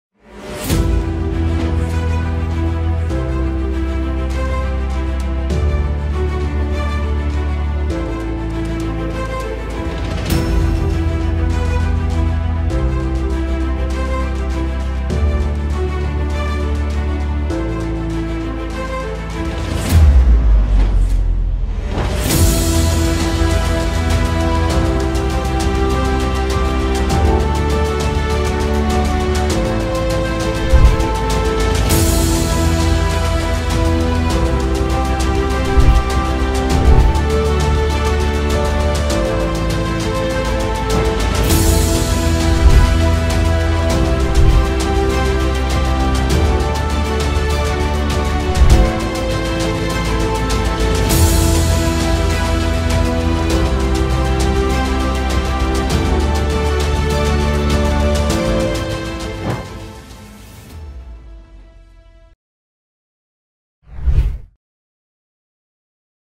برومو فعاليات لشركة هنوف القارات مؤتمر ومعرض خدمات الحج والعمرة 2024 بالمملكة العربية السعودية
لأعمال المونتاج والتصوير وكل ما يخص السوشيال ميديا
تواصل معنا من  خلال الواتس اب